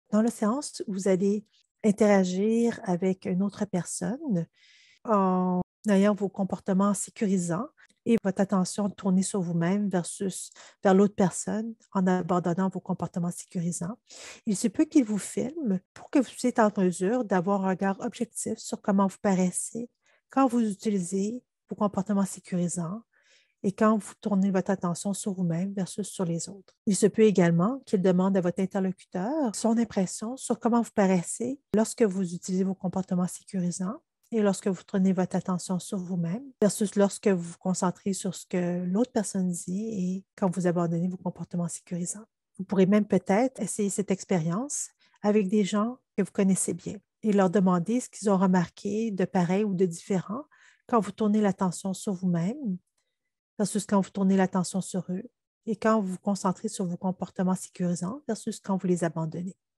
3. Rétroaction video
Capsule d'information portant sur le trouble anxiété sociale

Pour consulter le manuel du patient qui contient des explications et des outils pour gérer le trouble anxiété sociale

Cette présentation est uniquement destinée à des fins éducatives. Elle ne remplace pas un avis médical ou la recherche d'une aide professionnelle par le biais d'une thérapie ou d'un autre traitement.